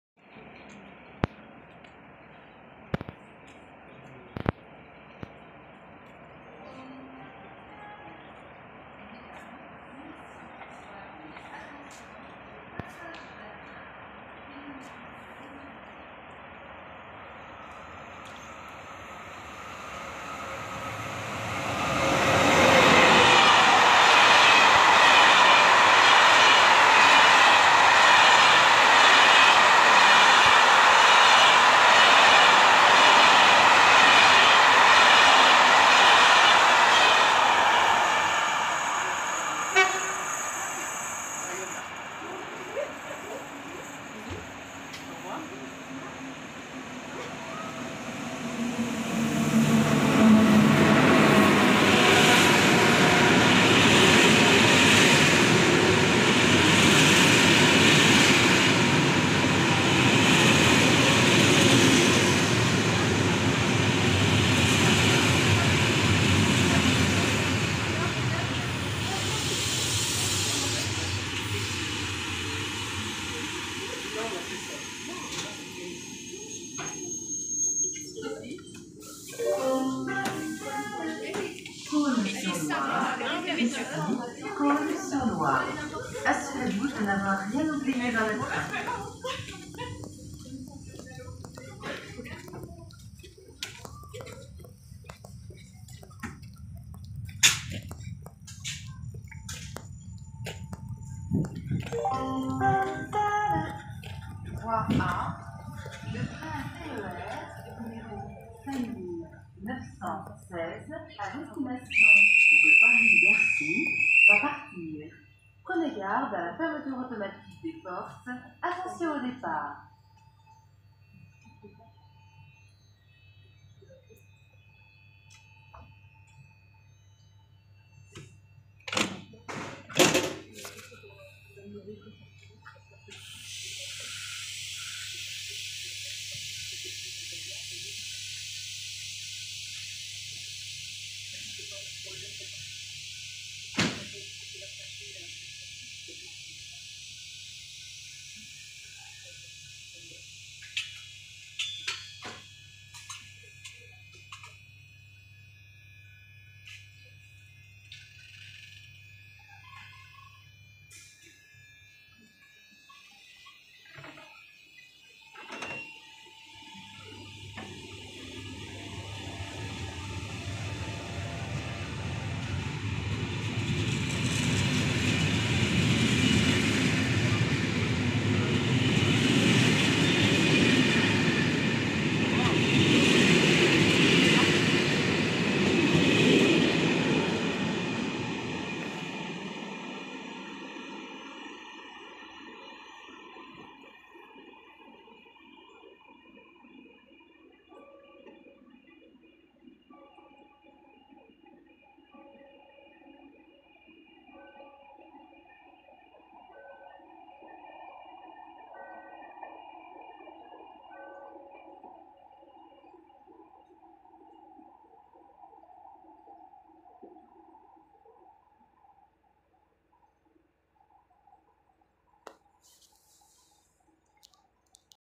Intercités TER Centre Val de Loire En Gare de Cosne-Cours-sur-Loire
Passage De BB 26044 Avec 14 Voitures Corail Intercités N 5973 Paris Bercy Clermont-Ferrand
0:45 Arrivée BB 22200 Avec 7 Voitures Corail TER Centre Val de Loire N 5916 En Provenance De Nevers
2:41 Depart TER Centre Val de Loire N 5916 Destination Paris Bercy